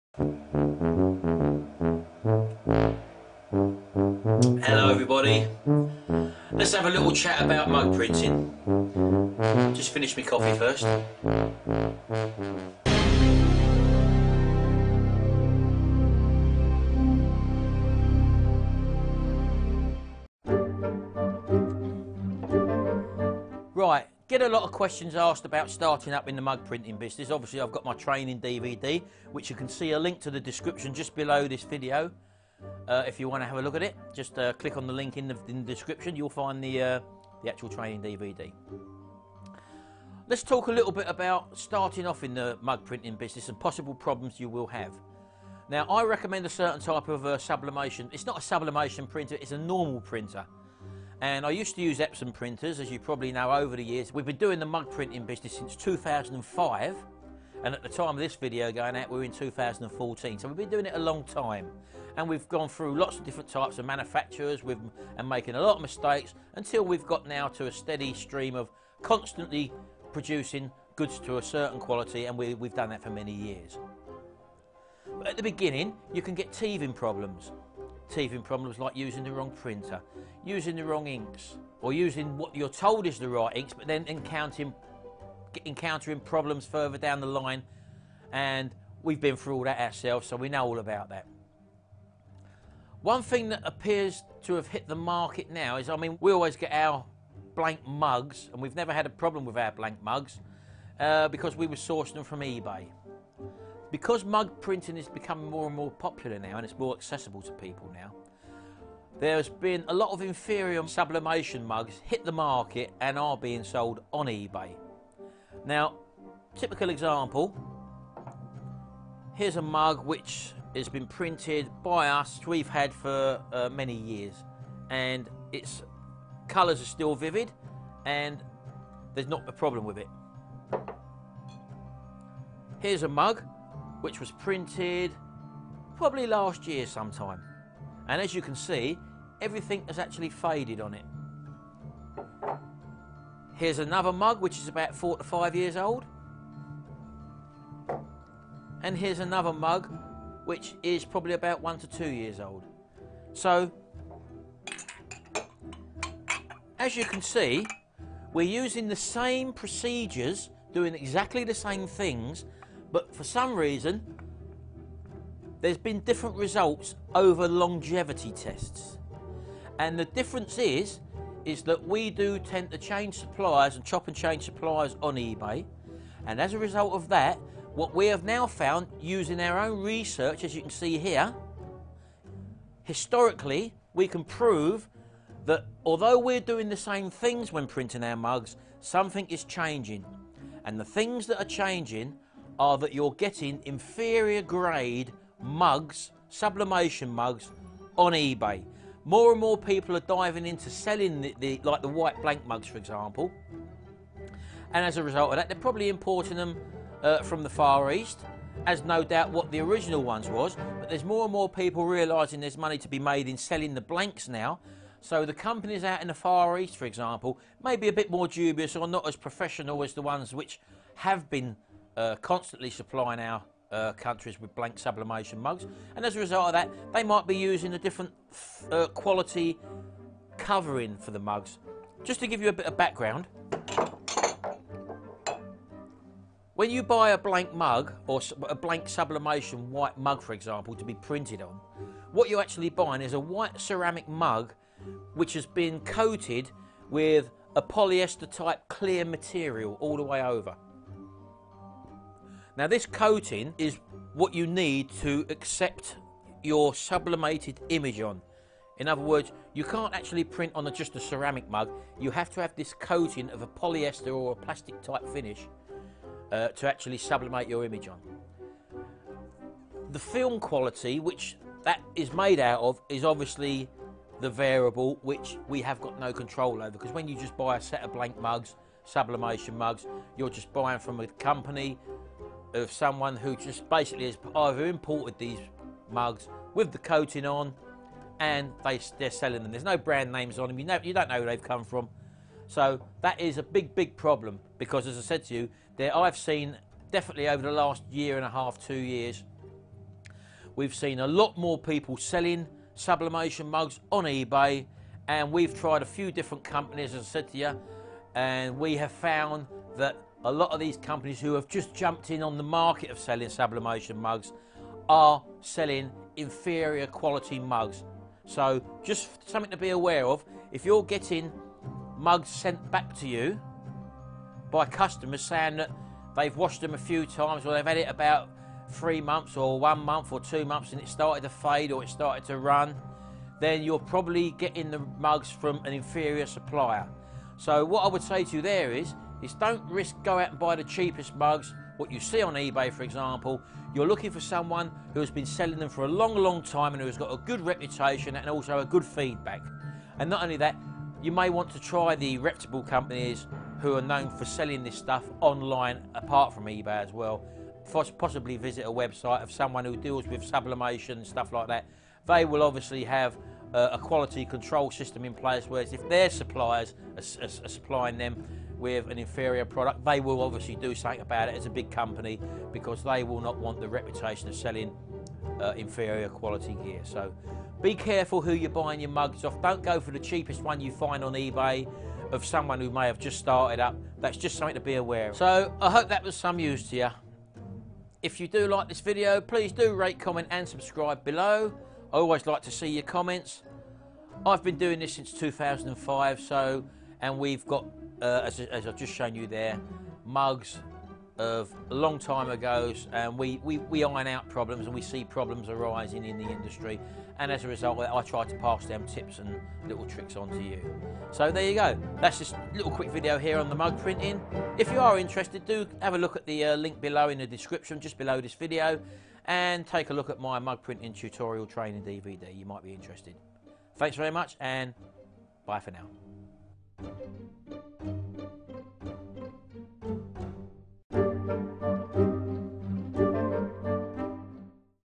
Mug Printing Beware Cheap Sublimation Mugs
Here is my mug printing training dvd.
Beware buying cheap sublimation mugs as they can have an inferior coating on them and could mean that your mug images fade in a sort time. In this video I show some examples that I have collected over the years to see what a poor sublimation mug produces.